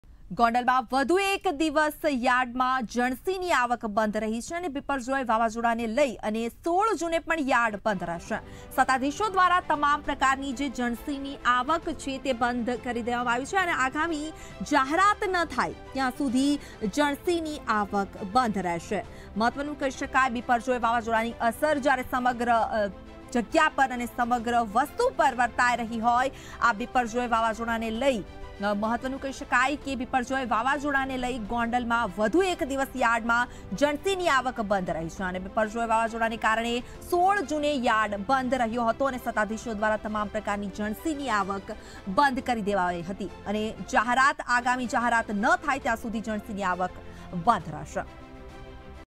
ગોંડલ: વધુ એક દિવસ યાર્ડમાં જણસીની આવક બંધ
બિપરજોય વાવાઝોડાને લઈને 16 જૂને યાર્ડ રહેશે બંધ
સત્તાધીશો દ્વારા તમામ પ્રકારની જણસીની આવક બંધ
આગામી જાહેરાત ન થાય ત્યાં સુધી જણસીની આવક બંધ